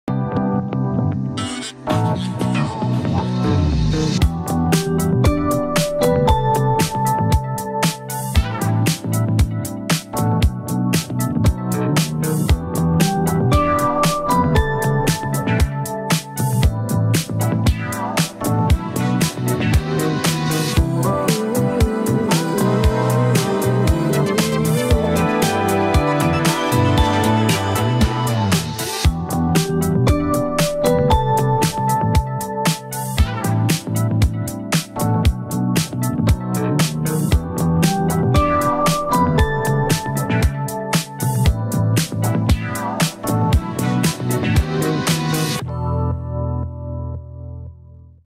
Hand Made Broom.
Coconut Brooms are made from coconut tree leaf which is suitable for cleaning rough ground and are ideal for stiff sweeping jobs such as sweeping fallen leaves 🍂and cleaning between stones etc. and is environment friendly material.